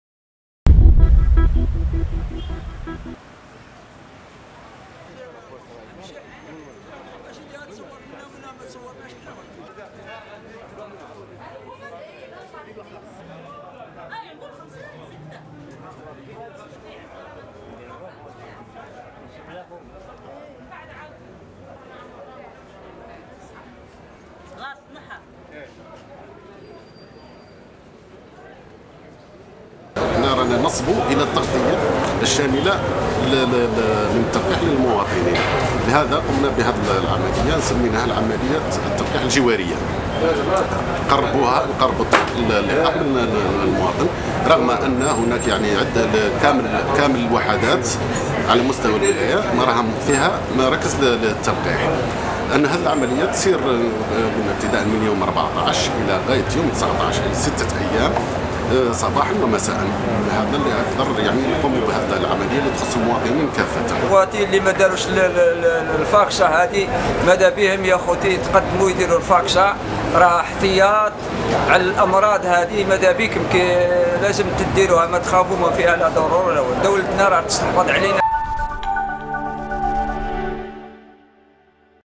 كوفيد-19: إطلاق حملة التلقيح عبر الفضاءات العمومية بورقلة
ورقلة (وأج) - أطلقت حملة التلقيح ضد فيروس كورونا عبر فضاءات عمومية بورقلة تحت شعار كلنا معنيون بالتلقيح ضد فيروس كورونا.